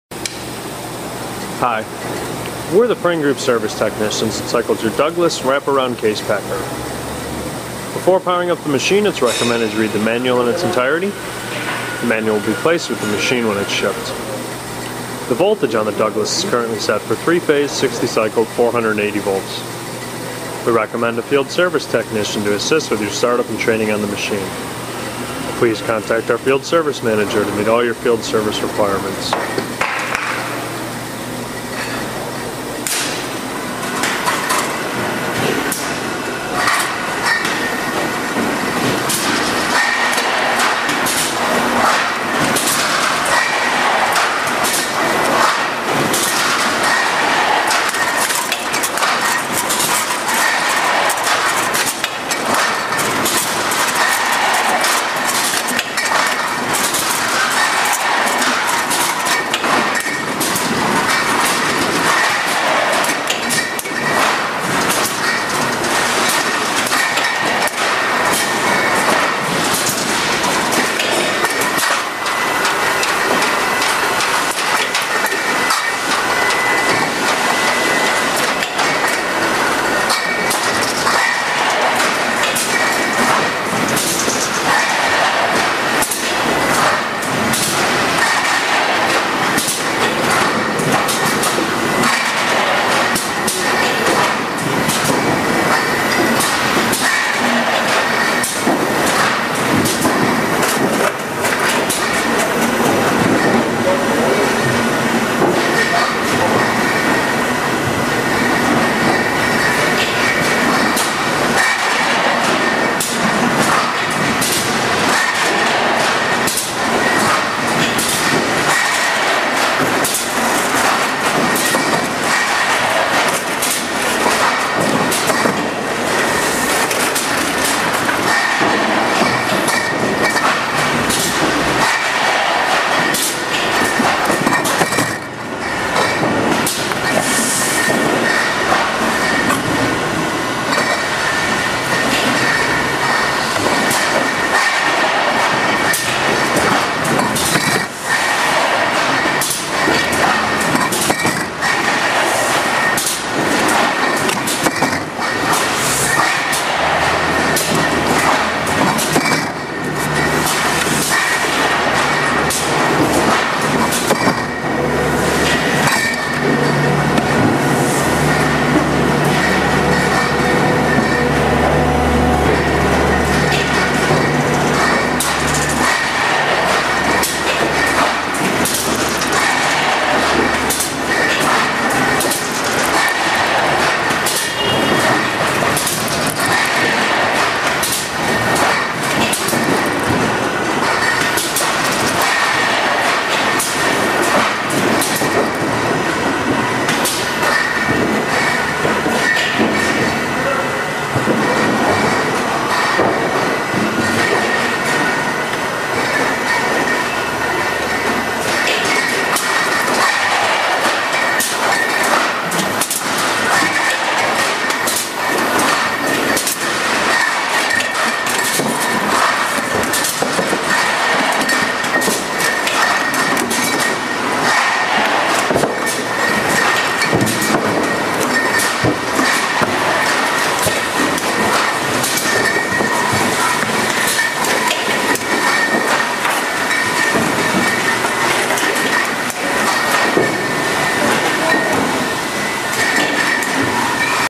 Douglas Machine Inc, WACP/TF-21, Tray Form Case Packer
To view this piece of machinery on our website click here;

Automatic, intermittent motion, wrap around case packer/tray former rated 5 to 18 cpm - depending on materials and application. Equipped with a 96" long x 20" wide infeed conveyor, transfer section, 84" long blank case magazine, reciprocating pick and place, 4 chain lug system set on 21" centers with 4" wide x 3" high lugs, flap plows and closers, hot glue system side compression units, push button control panel with visual display, A/B PLC, and interlocking Lexan safety guarding. Mounted on height adjustable legs. Last running a 10" long x 15" wide x 6" high case.